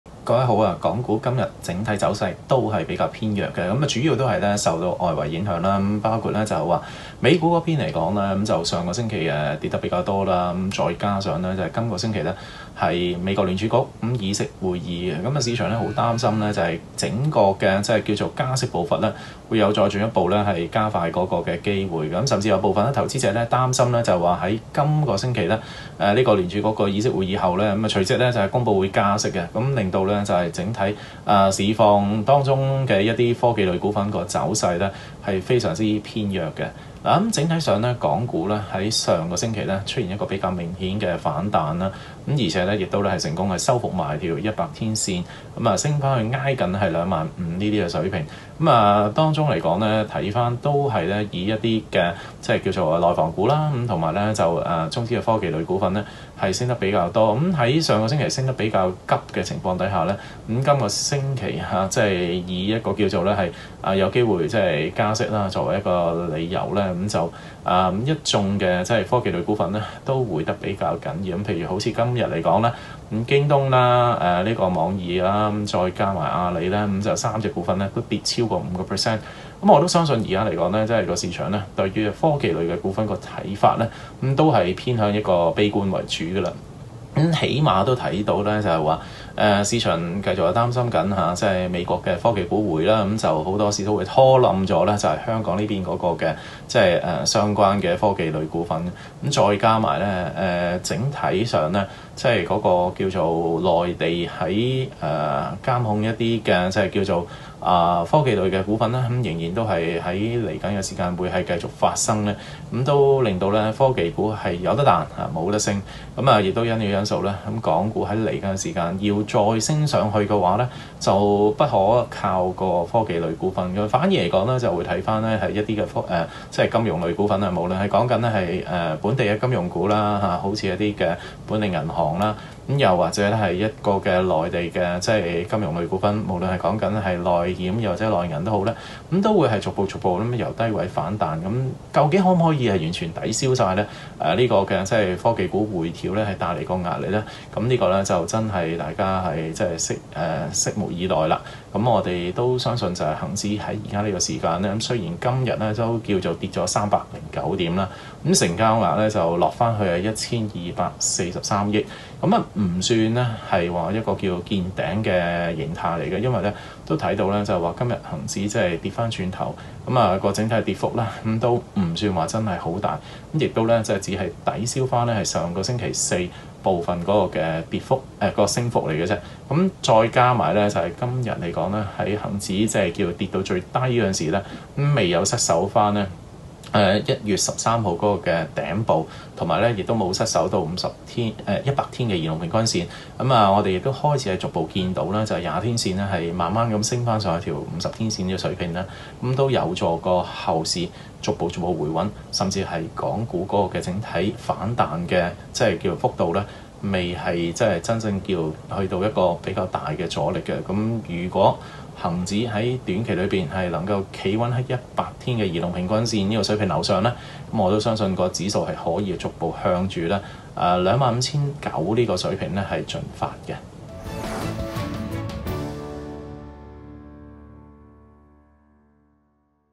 彭偉新專家短評｜大市未見頂 先守住一個關鍵指標……
美國聯儲局議息前，港股走弱。彭偉新認為市場憂慮加息步伐加快，科技股偏悲觀，至於金融股能否抵銷科技股跌勢仍需時觀察，但技術走勢上恒指未出現見頂形態，如果可以守住一個指標，則仍可再升半成……

彭偉新#專家短評#港股#恒指#聯儲局議息#科技股#金融股#技術走勢